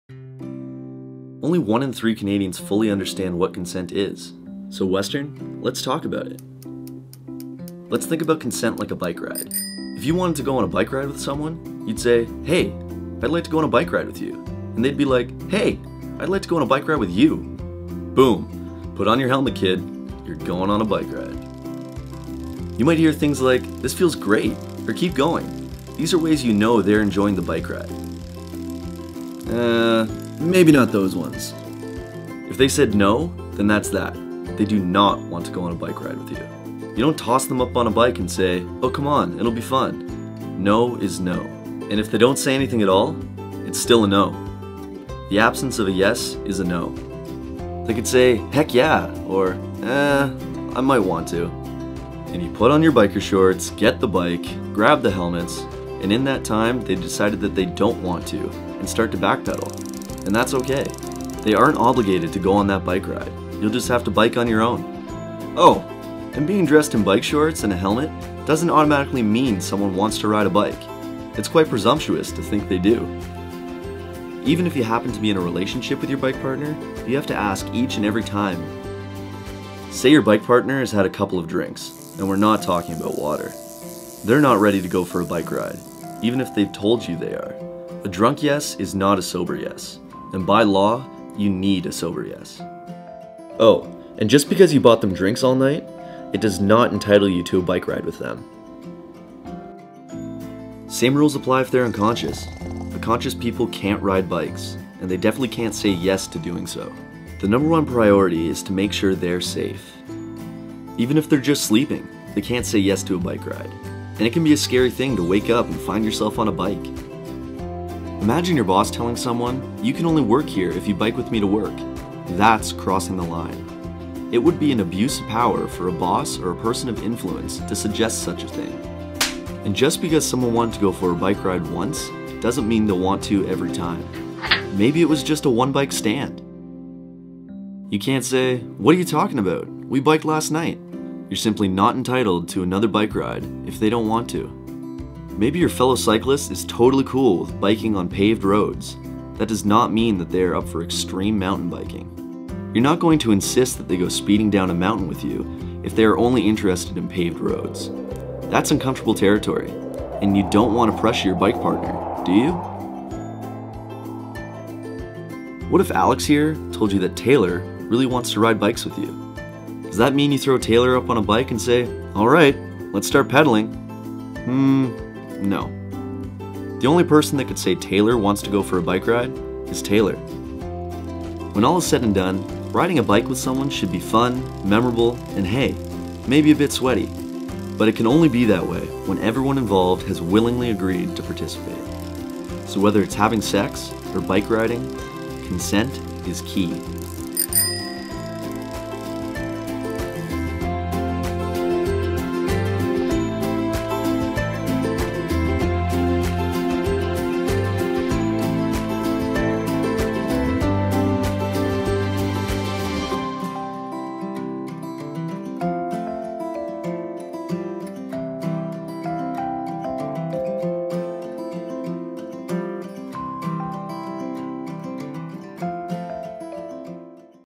Cycling Through Consent
"Only 1 in 3 Canadians fully understand what sexual consent is."

So let's talk about it. This animated video takes on a fun and simple approach to looking at consent; exploring what consent is and what it does and does not look like.

Learn more about sexual violence prevention, resources and education at Western

Uploaded by: Communications and Public Affairs (MH)